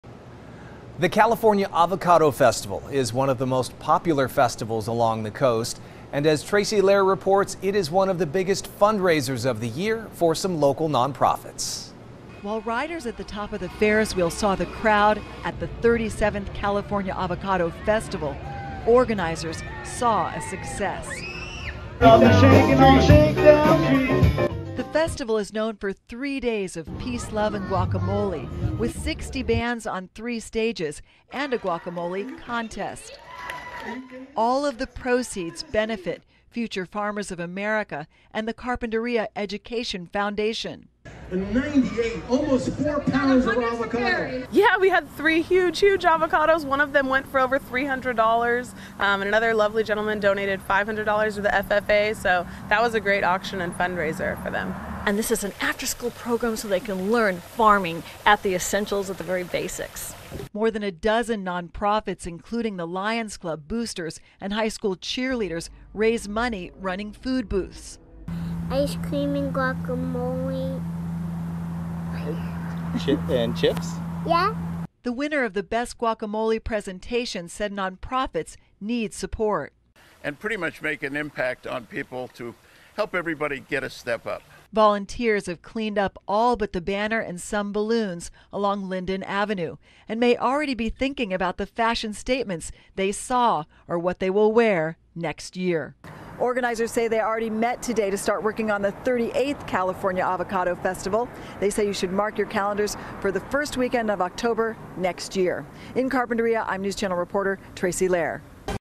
California Avocado Festival helps nonprofits raise funds
California Avocado Festival helps nonprofits raise funds [(977717)] | Read the full story